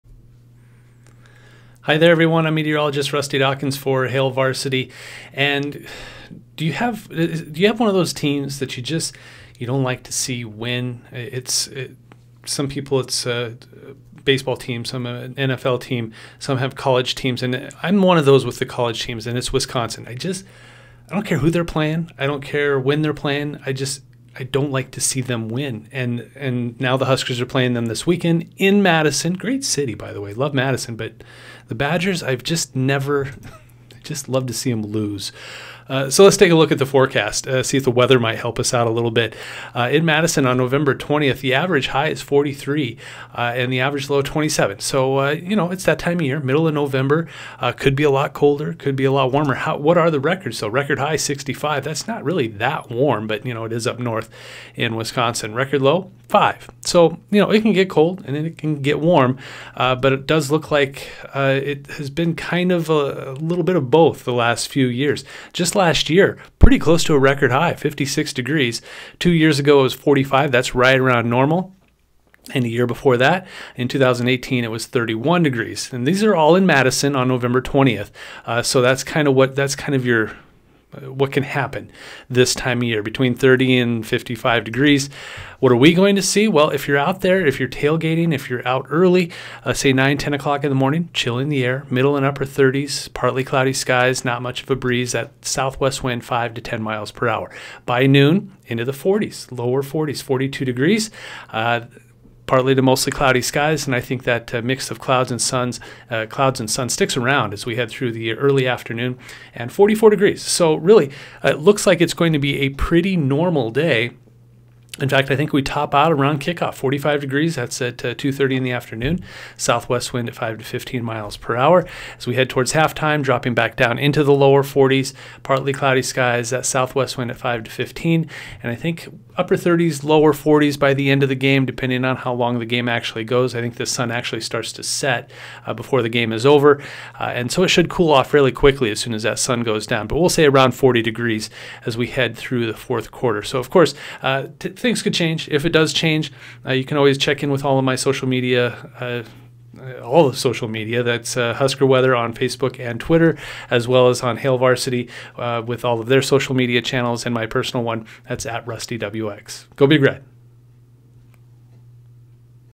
Nebraska-Wisconsin 2021: Game Day Forecast
What can you expect weather-wise for Nebraska's matchup with Wisconsin on Saturday, Nov. 20?

Going up north in November can be a little tricky, however Saturday looks pretty nice for Nebraska's matchup with Wisconsin.